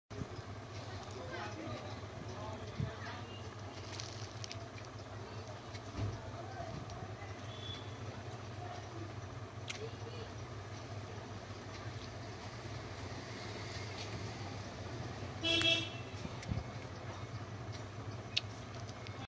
Driver forget to change Gear
Funny guy forget to change gear